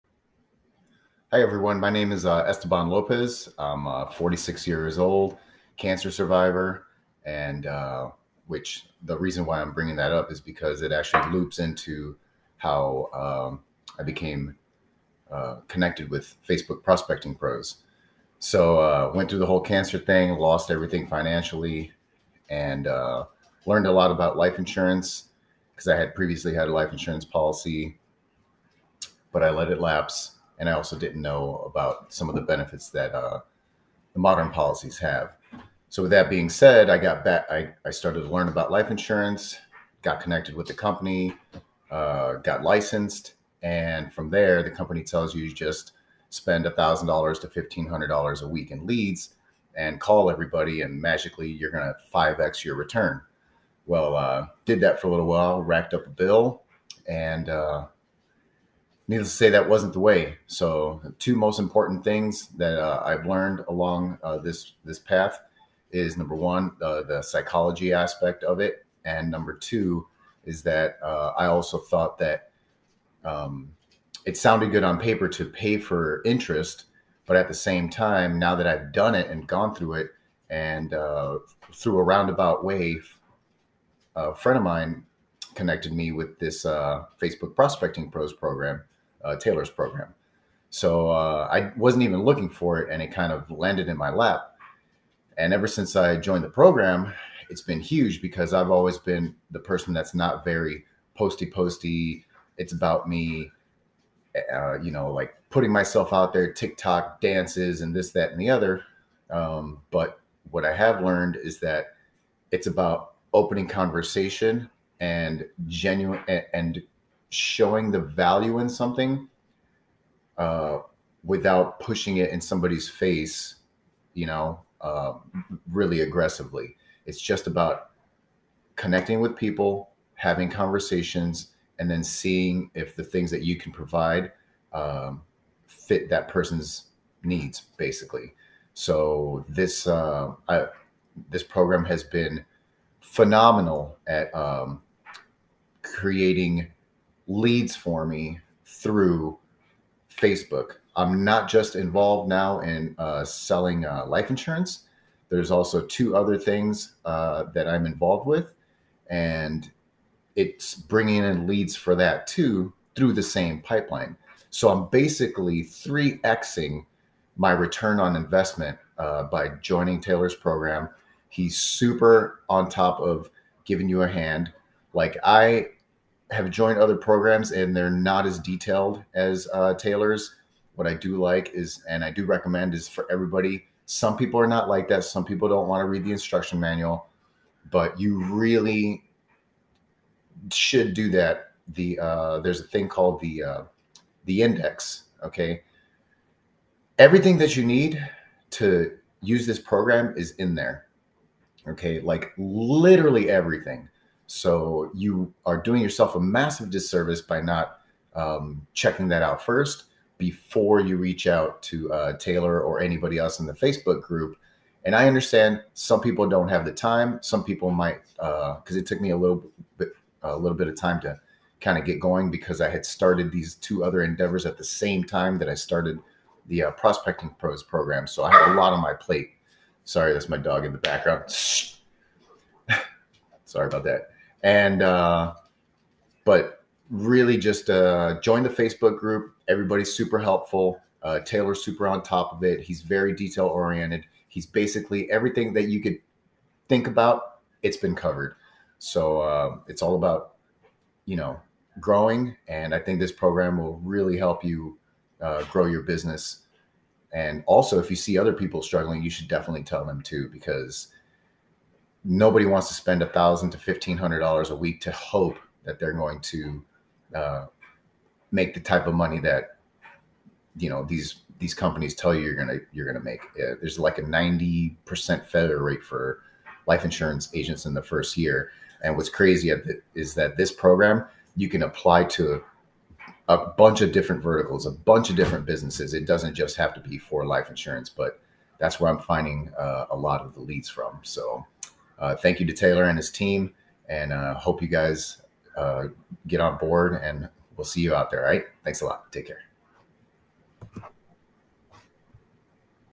Esteban Lopez - Facebook Prospecting Pros Testimonial
To everyone watching, let me welcome Esteban Lopez!

Listen as Esteban explains his experience and all the insight he's gained inside of the Facebook Prospecting Pros Academy.